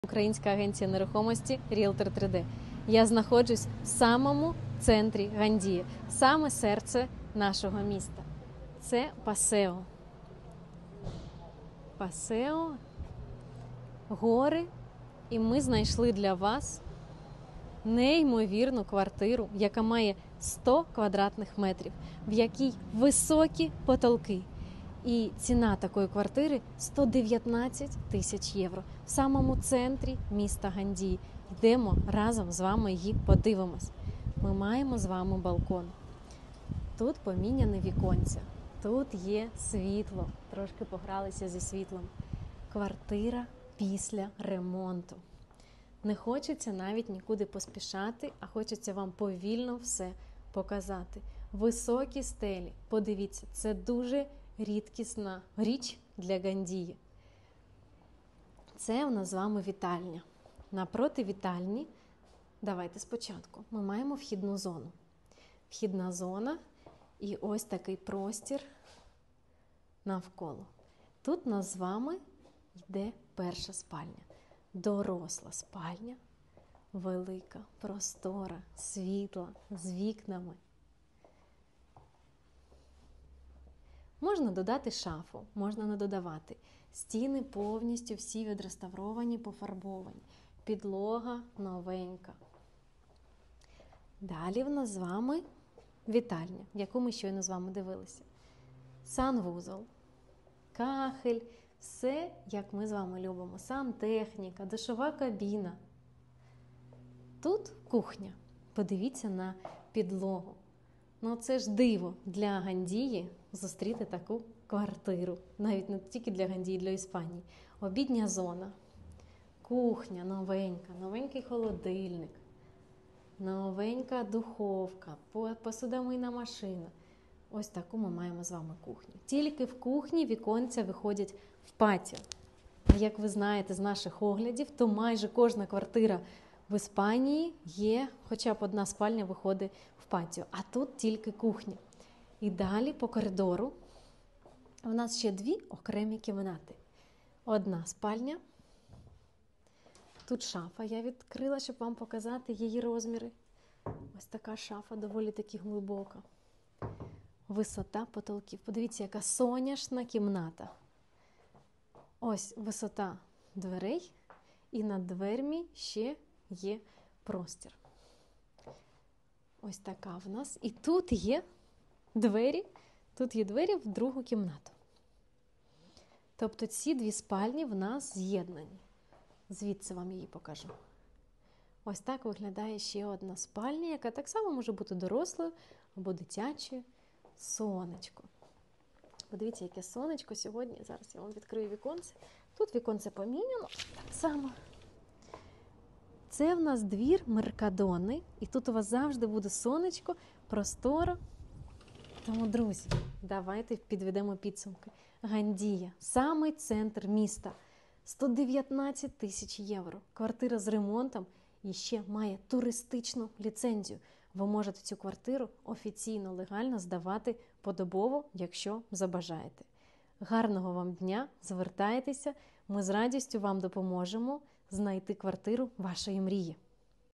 Неймовірна квартира 119.000 € 100м2
✅ 3 кімнати + вітальня + кухня
✅ 119.000 € вартість квартири
Переоформлення:
- 10% податок на нерухомість
- ріелторська комісія
- нотаріальне переоформлення
- страхування нерухомості
Точну суму переоформлення «під ключ» вам вирахує персональний менеджер!
Орієнтовна вартість оренди становить 850 € на місяць!

📏 Площа - 100 м2
🚽 Санузлів - 1
🏢 Поверх - 3/3 укр.)

🏠Адреса: Gandia,
Passeo Germanies

✅Gandia - це місто недалеко від Valencia, яке розташоване поряд з найкращими 🏖️пляжами в Іспанії!
🛬Ваш запасний аеродром на будь-який час

З повагою, УКРАЇНСЬКА 🇺🇦 АГЕНЦІЯ НЕРУХОМОСТІ В ІСПАНІЇ - “REALTOR3D”
Іспанія, місто Гандія, paseo Germanies 93